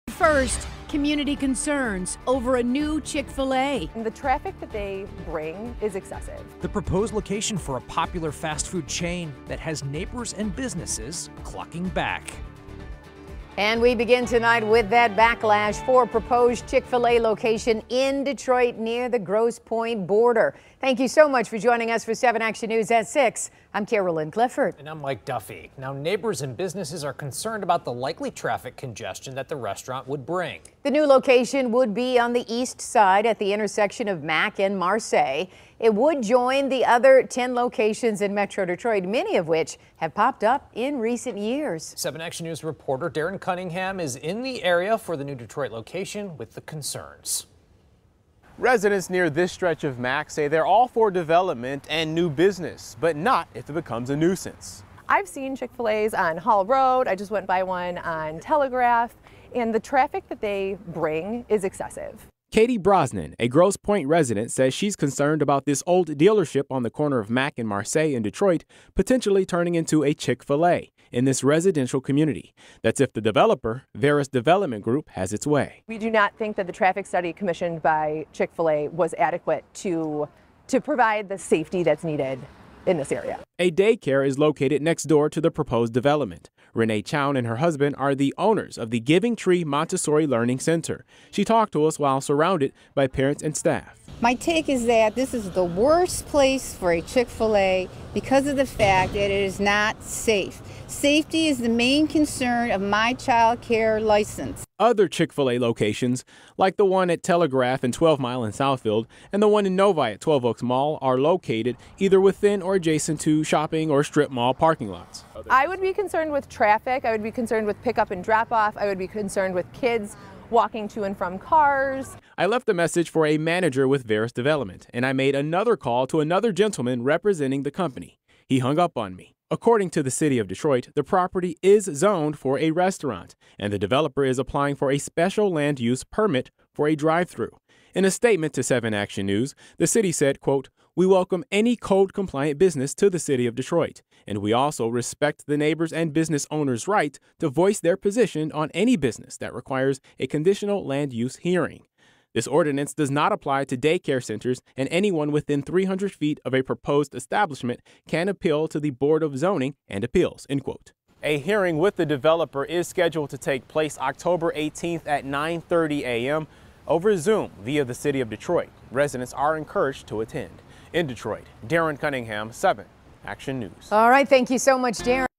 Residents, businesses concerned about traffic from proposed Chick-fil-A in Detroit
Residents and business owners near Mack Avenue and Marseille Street in Detroit say they aren't excited about a proposed Chick-fil-A development.